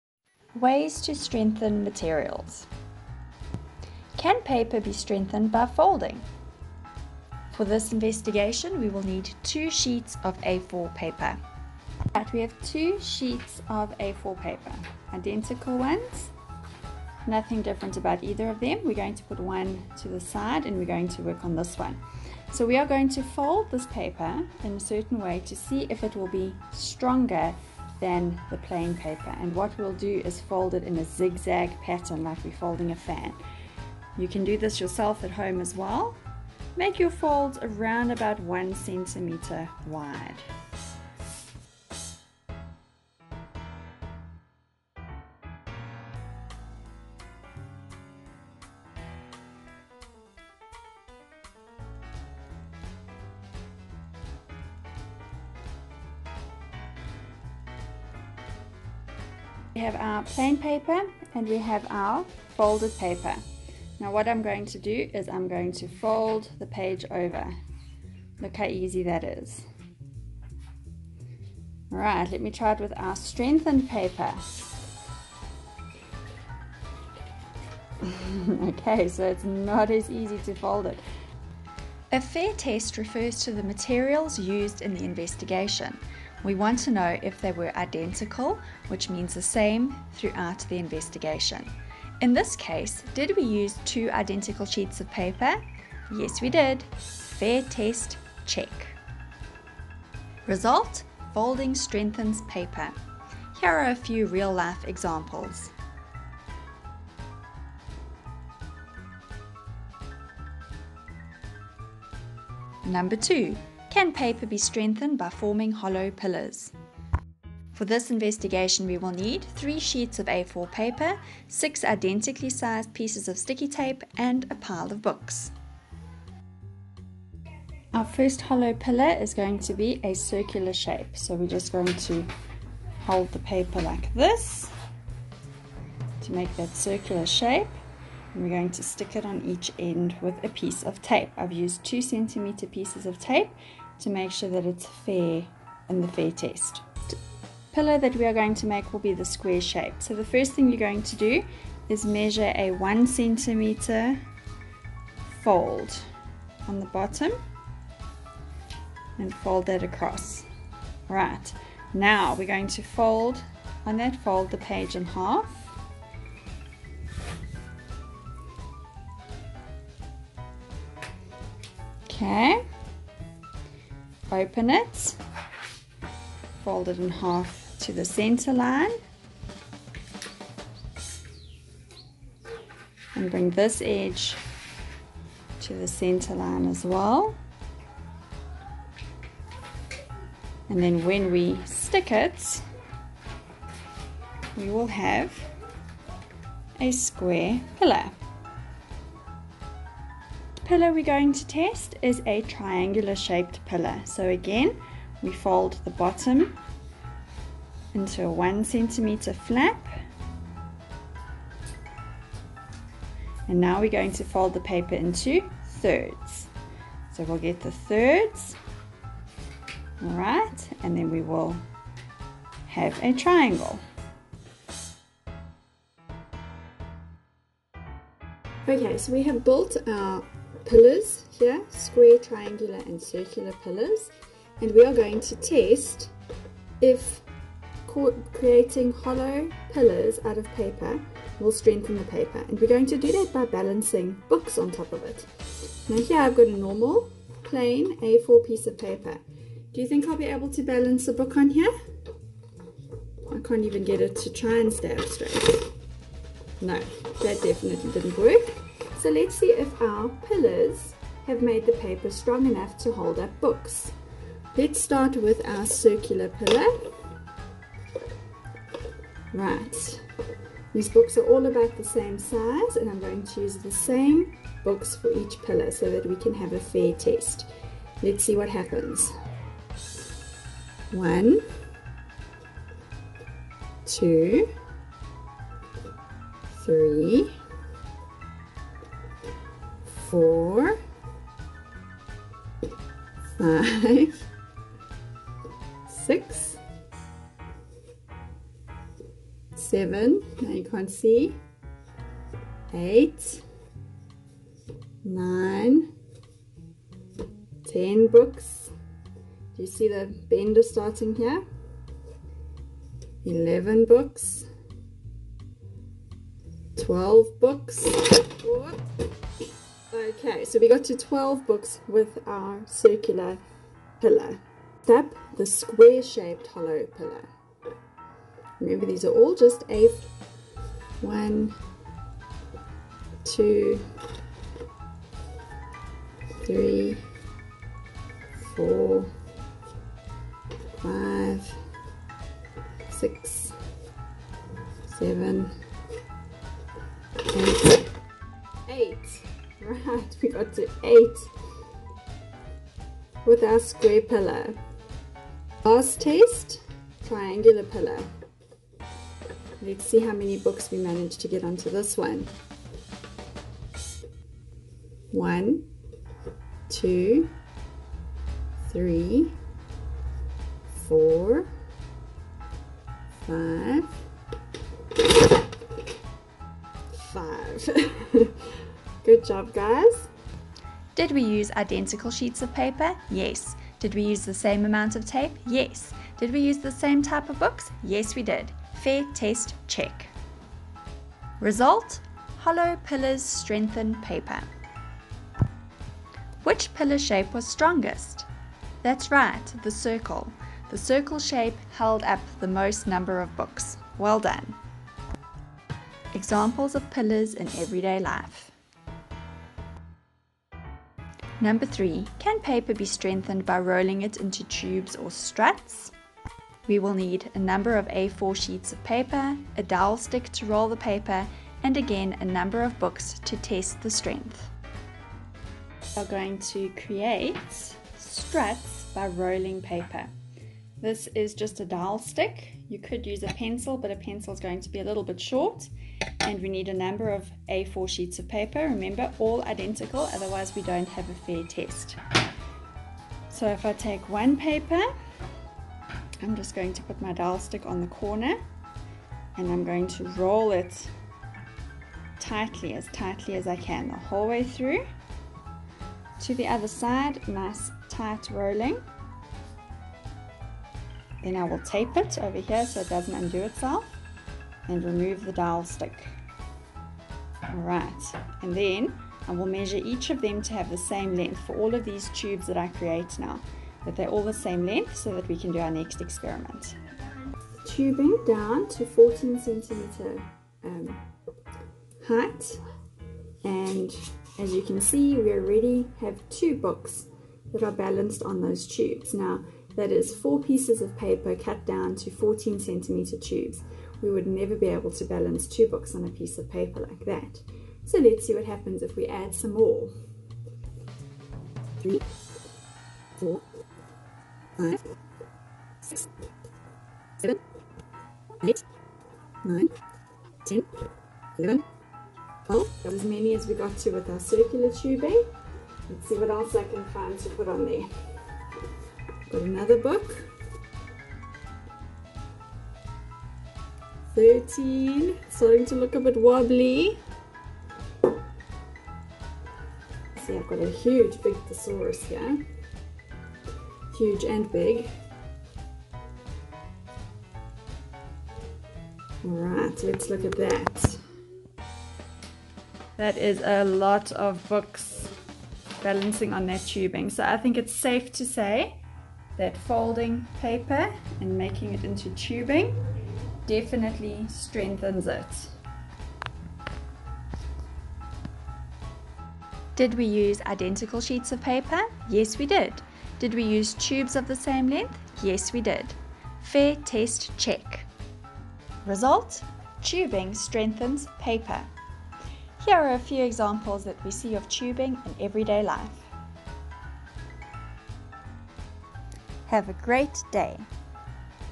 Grade 4: Ways to strengthen materials
Basic intro for Grade 4. Videos filmed at home during lockdown 2020. Images may be subject to copyright.